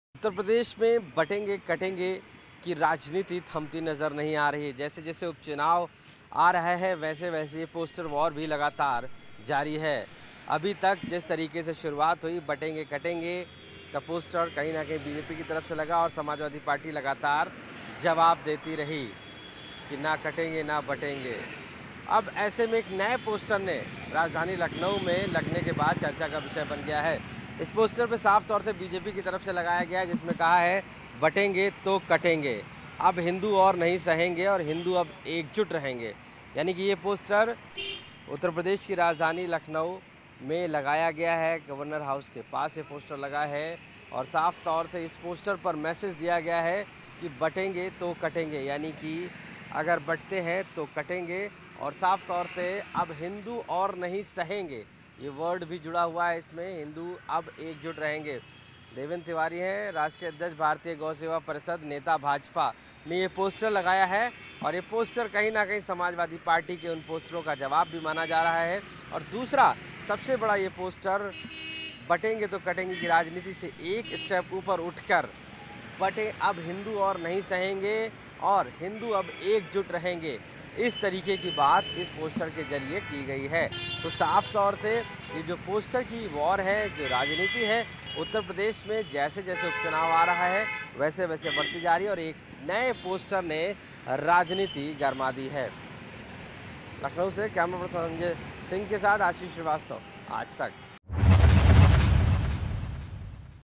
UP News: BJP Samajwadi Party का नया पोस्टर 'ना बटेंगे, ना कटेंगे, एक है और एक रहेंगे' | Aaj Tak
उत्तर प्रदेश में उपचुनाव के बीच बीजेपी और समाजवादी पार्टी के बीच पोस्टर वार जारी है. बीजेपी ने 'बटेंगे तो कटेंगे' और 'हिन्दू अब एकजुट रहेंगे' संदेश वाले पोस्टर लगाए हैं. सपा ने जवाब में 'ना कटेंगे ना बटेंगे' संदेश दिया है. ये पोस्टर उत्तर प्रदेश की राजधानी लखनऊ में लगाए गए हैं. इस पोस्टर वार के पीछे की राजनीति क्या है ?